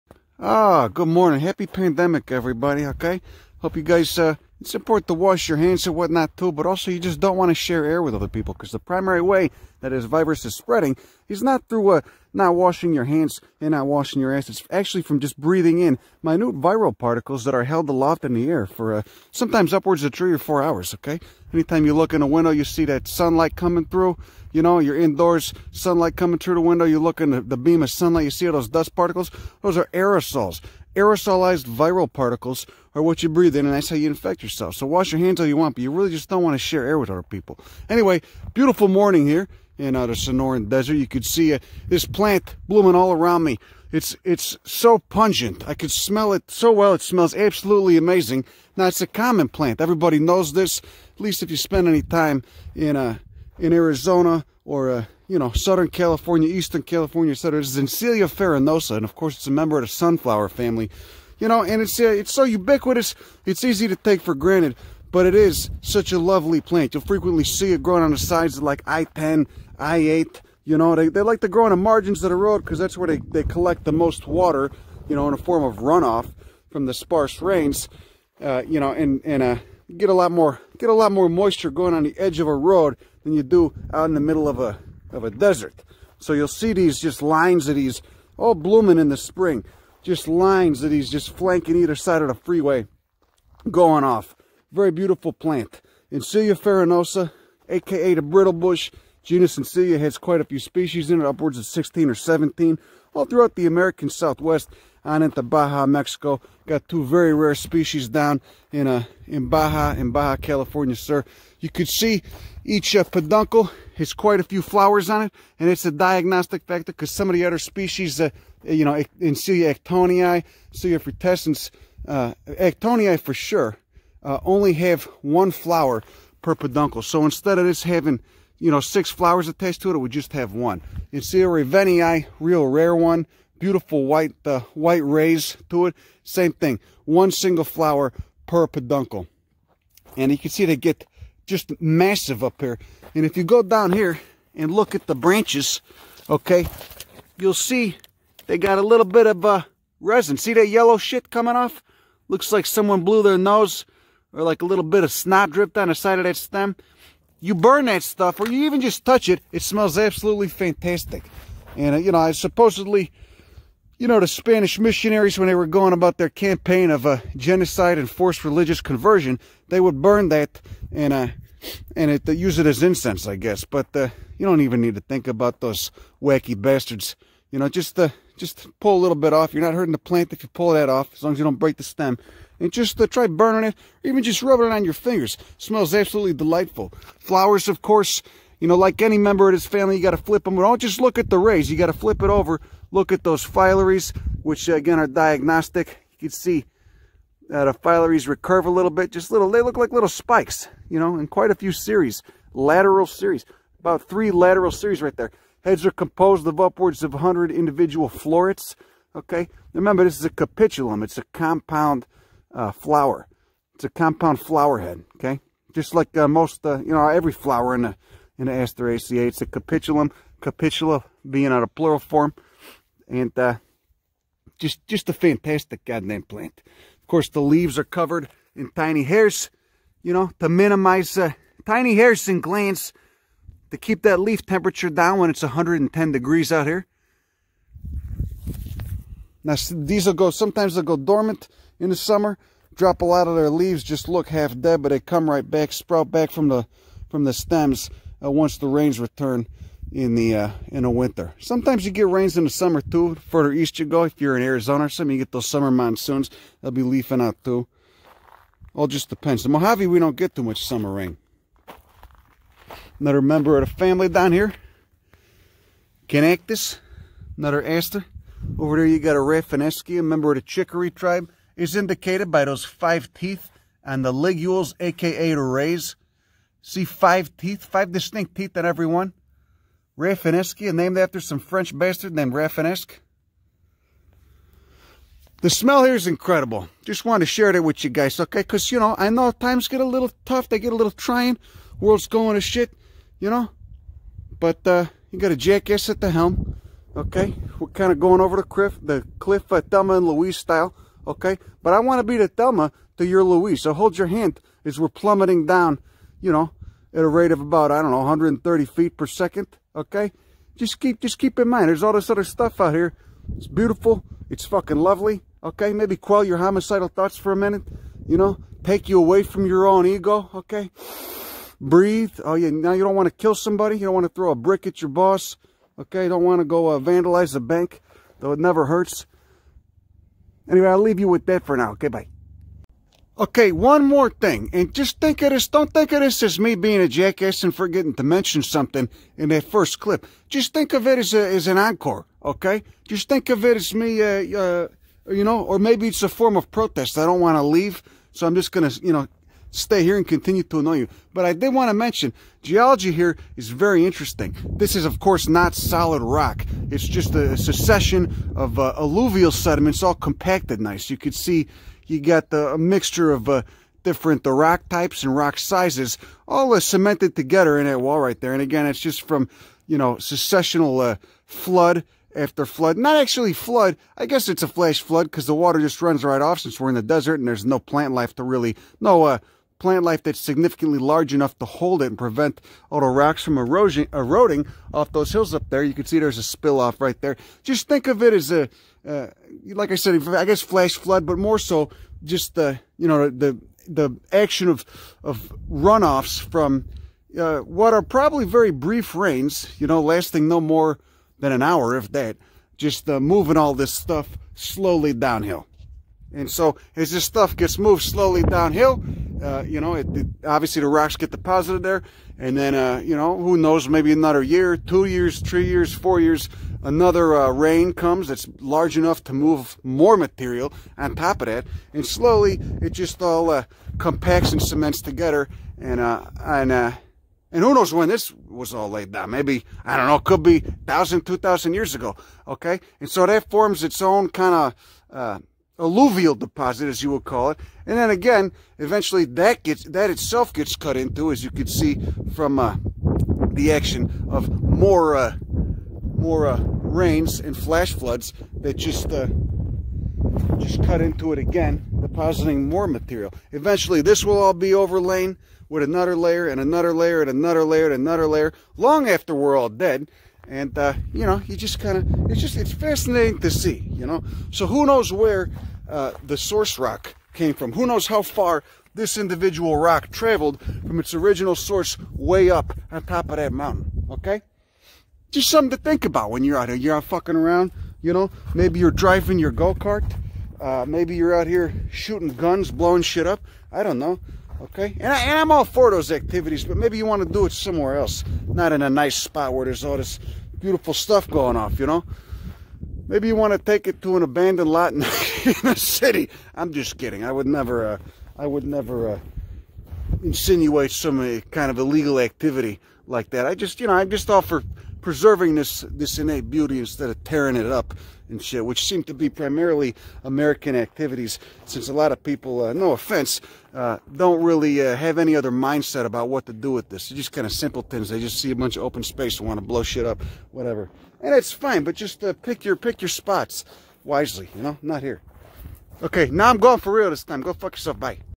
Coronavirus PSA by Encelia farinosa
This video demonetized for mention of Coronavirus (it's worth it). So if you see AN obnoxious ad (they're all obnoxious) on this video, let the CPBBD complaint department know.

In these anxiety-inducing times, it's important to remember the world is still beautiful and what the shit. At least, it is if you find yourself in a remote desert wash filled with thickets of brittlebush, Encelia farinosa. Join us for a CPBBD short to light your ass up with the sights and sounds of this member of the sunflower family blooming against a backdrop of tens of thousands of year's worth of flash floods and alluvial deposits.